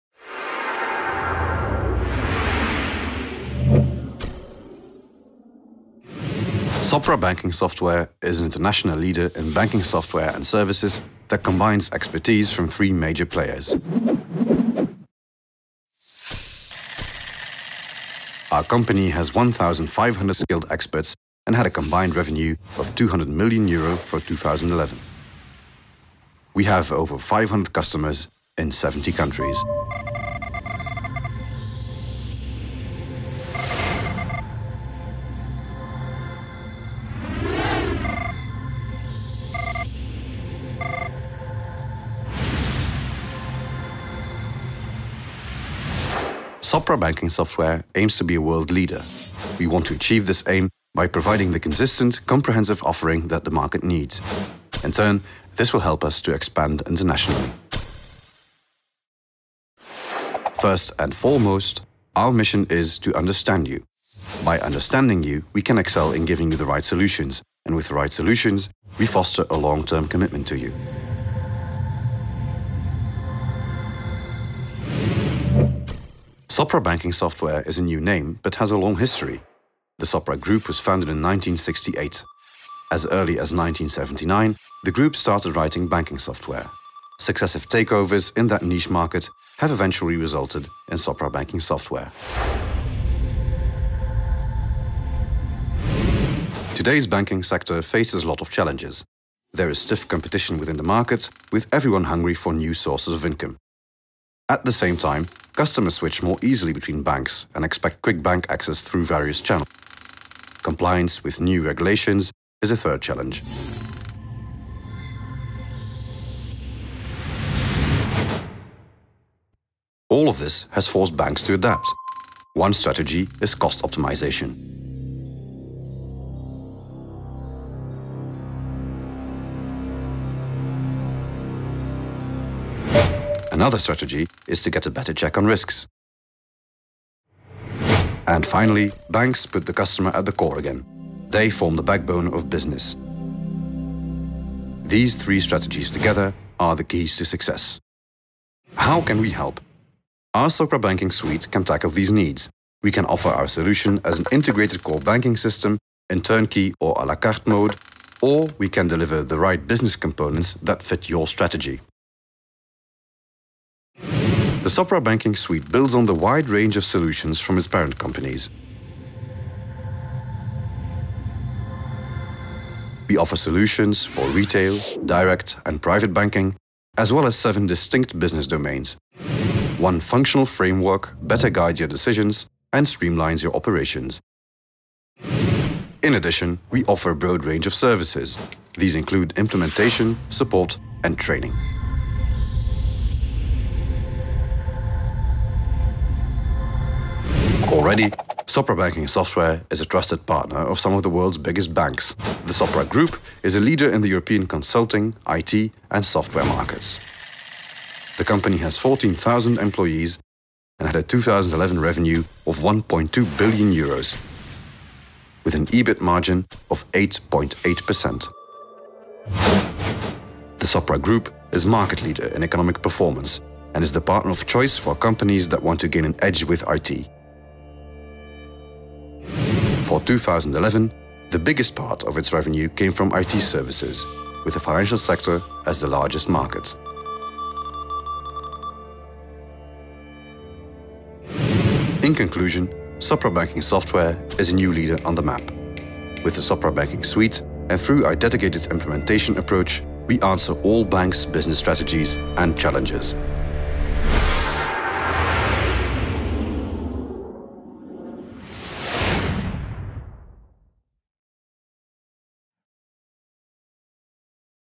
Sopra Banking Software, Corporate Presentation
Sopra Banking Software is a global leader in software and IT services for banks and financial institutions. Its solutions help over 500 customers in 70 countries be more efficient, agile and responsive to business challenges. The market has long recognized the company's constituent parts - Sopra Group's financial software business, Callataÿ & Wouters and Delta Informatique - as solid players with a wide range of proven products. Worldwide, Sopra Banking Software employs 1,500 skilled experts. The company is a subsidiary of the Sopra Group -- a leader in consulting, IT services and software development in Europe -- which employs more than 14,000 people and generated a pro forma turnover on 1,2 billion euro in 2011.